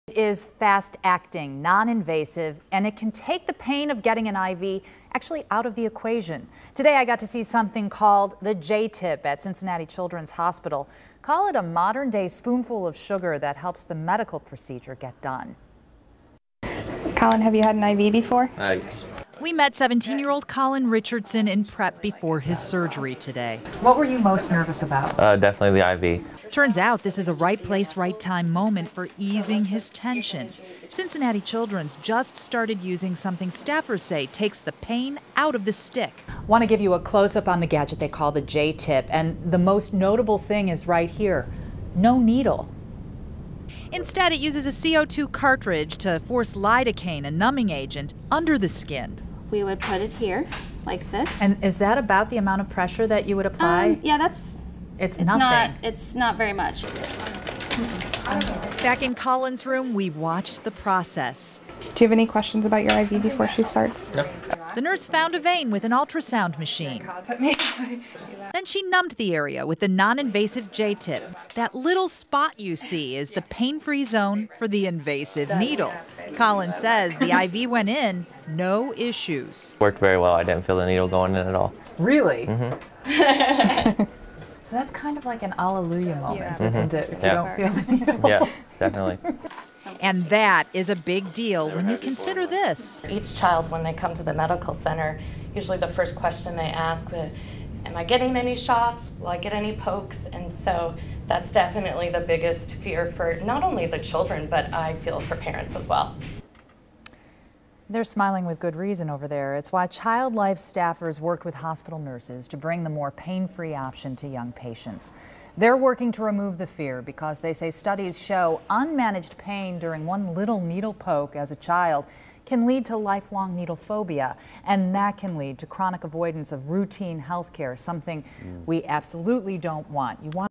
J-Tip reduces needle anxiety
Cincinnati Children's Hospital Medical Center is now using J-Tips, non-invasive tools that numb an area prior to an IV.